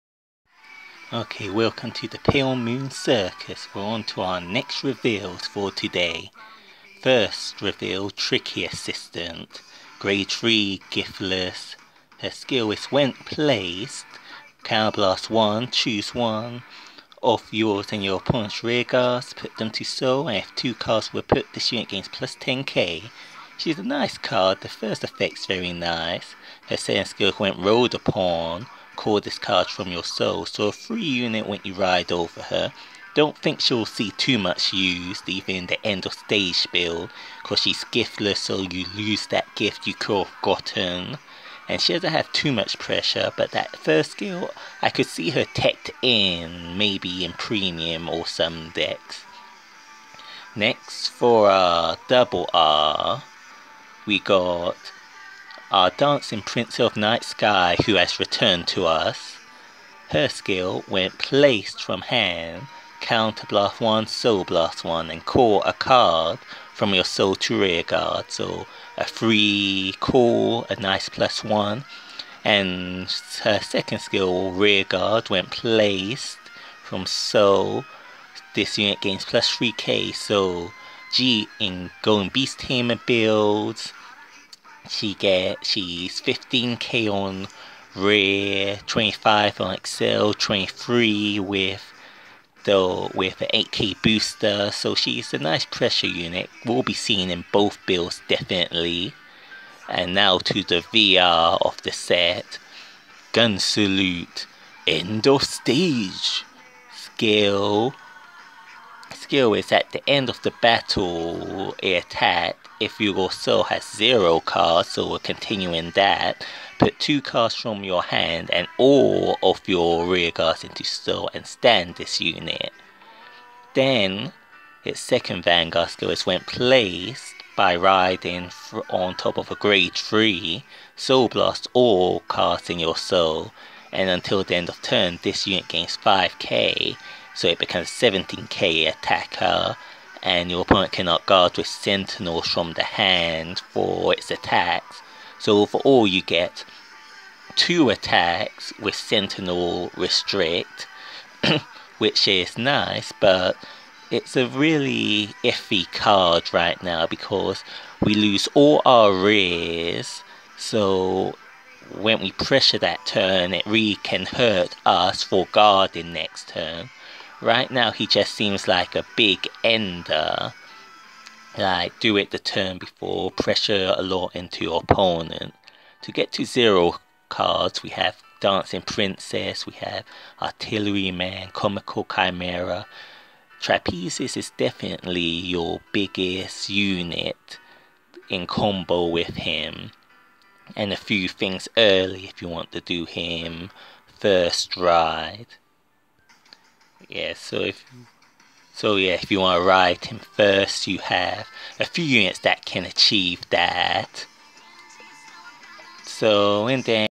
Circus Time cardfight vanguard pale moon COTD stream reveals End of stage and others
Just review on today's stream reveals.
I feel end of stage could be better if we get units that self call when pushed to soul